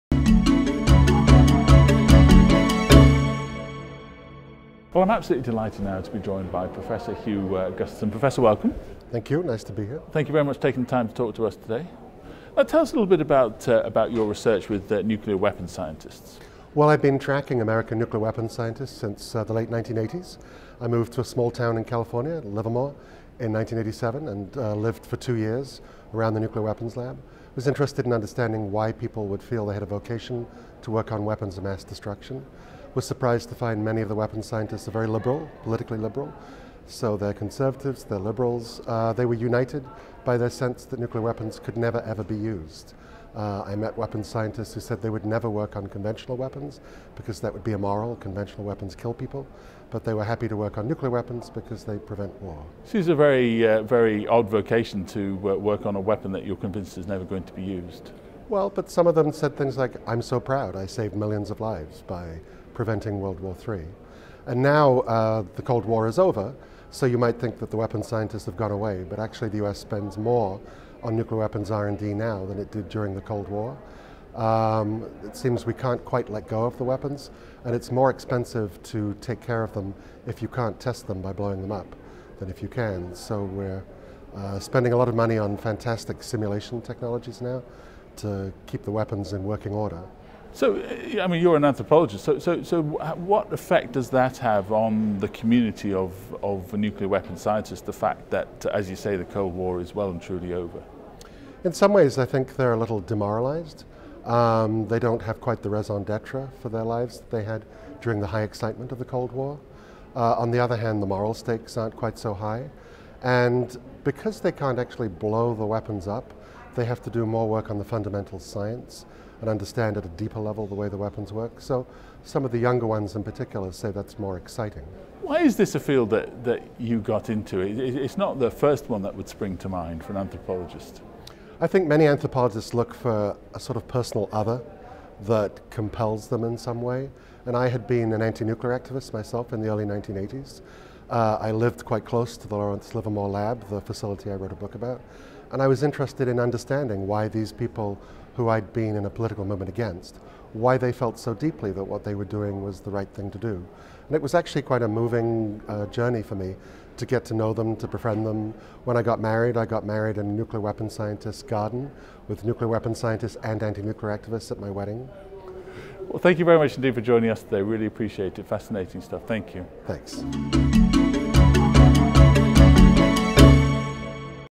Unuseable Weapons - Attitudes of Scientists Working on Nuclear Weapons
Anthropology TV spoke to Hugh Gusterson Ph.D. who's worked for many years looking at the attitudes of scientists working on nuclear weapons following the cold war.